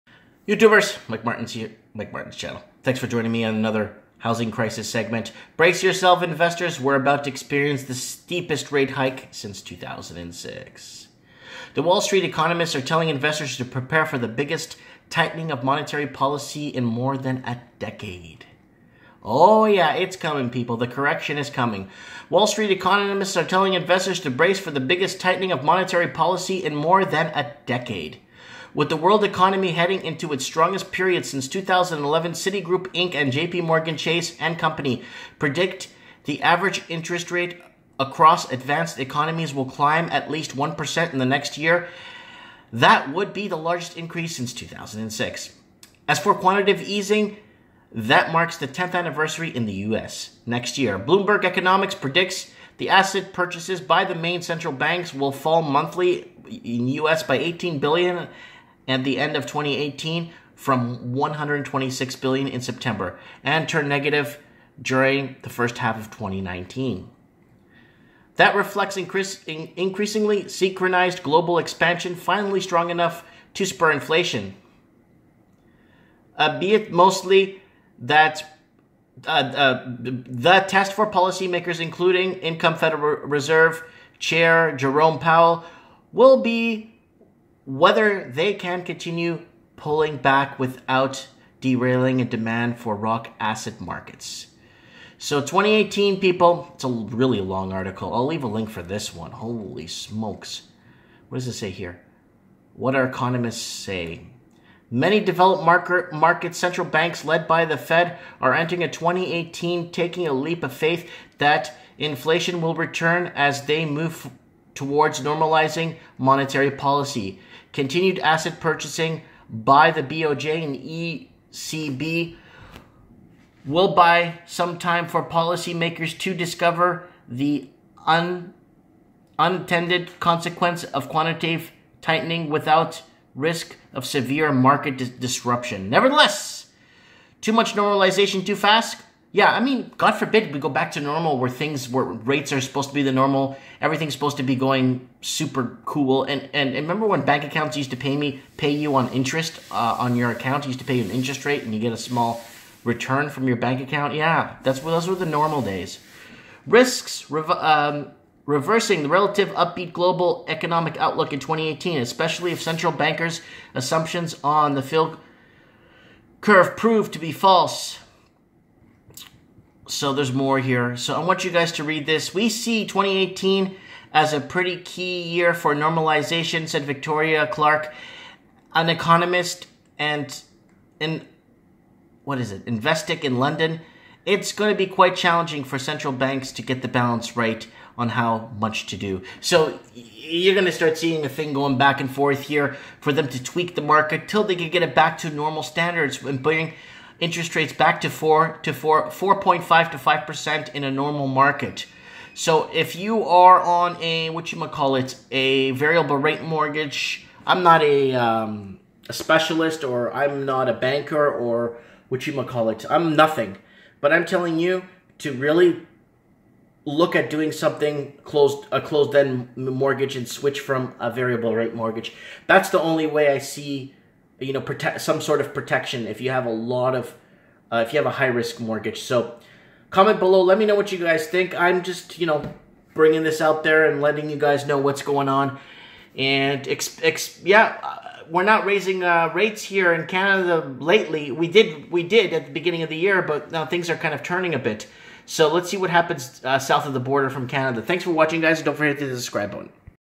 Rates are Finally Going UP ! to Curb Against this Crazy FOODFLATION and Crazy Housing Prices
MY OPINION:  This is a Much needed move to start bringing prices down to earth ! cities are abandoning with a youth mas exodus and talent moving south of the border .

Brace yourselves, investors: We're about to experience the steepest rate hikes since 2006

With the world economy heading into its strongest period since 2011, Citigroup Inc. and JPMorgan Chase & Co. predict average interest rates across advanced economies will climb to at least 1 per cent next year in what would be the largest increase since 2006.

As for the quantitative easing that marks its 10th anniversary in the U.S. next year, Bloomberg Economics predicts net asset purchases by the main central banks will fall to a monthly US$18 billion at the end of 2018, from US$126 billion in September, and turn negative during the first half of 2019.